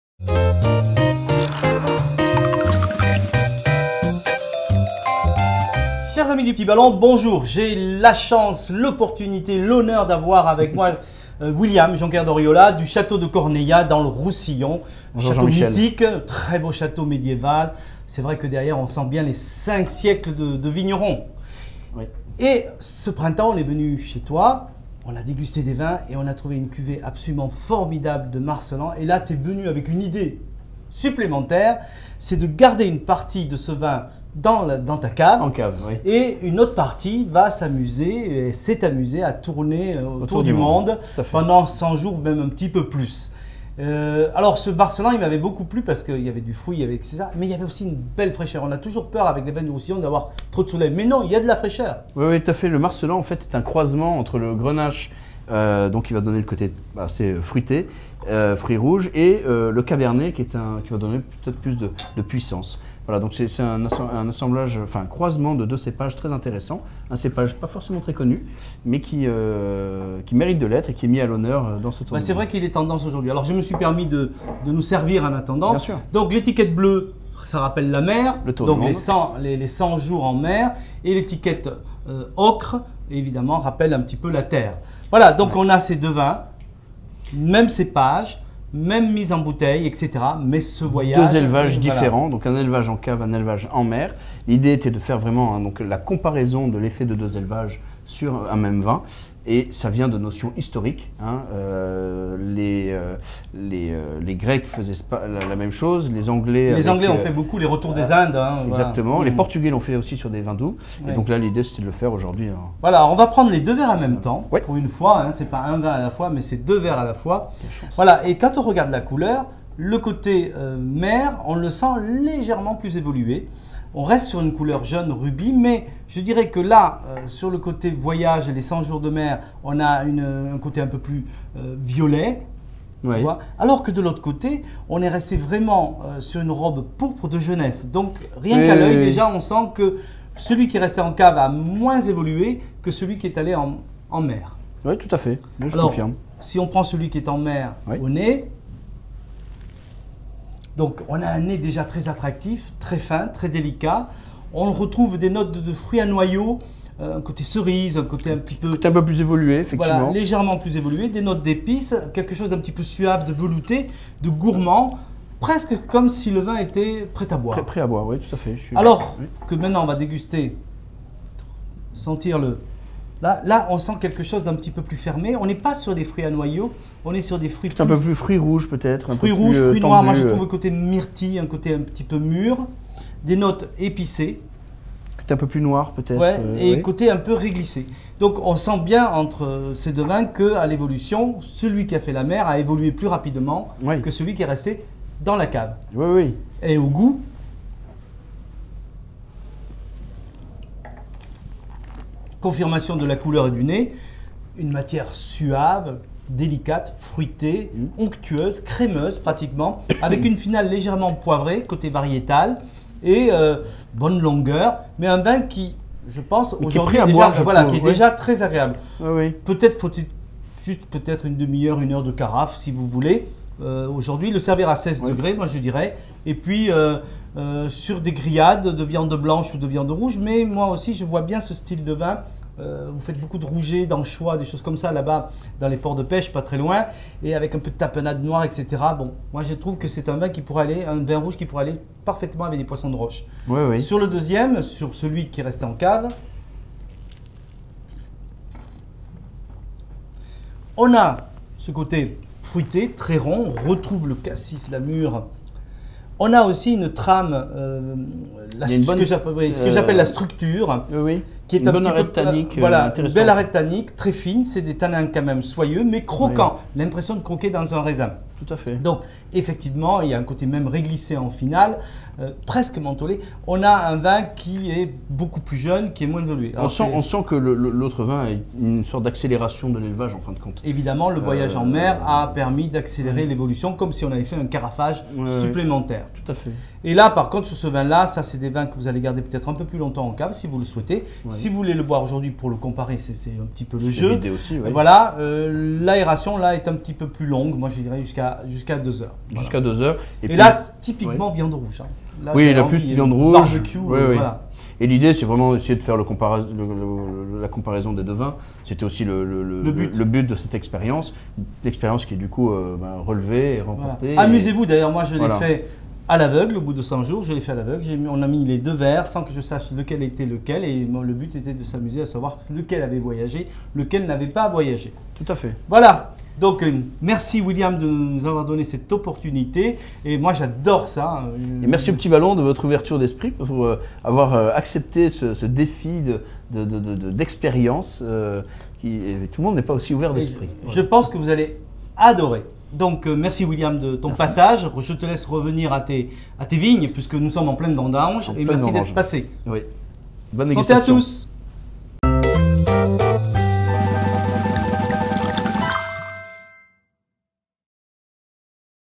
100 jours en cave vs 100 jours en mer // Dégustation Le Petit Ballon
Un vin, deux destins

Une légende de marin veut qu'un vin ayant voyagé en bateau vieillirait différemment d'un vin sagement gardé en cave. On a voulu en avoir le coeur net ! Jean-Michel Deluc et William du domaine Jonquère d'Oriola dégustent les deux bouteilles de l'opération.

Sur cette chaîne, retrouvez les dégustations des vins que notre équipe de passionnés dénichent pour vous dans le monde entier.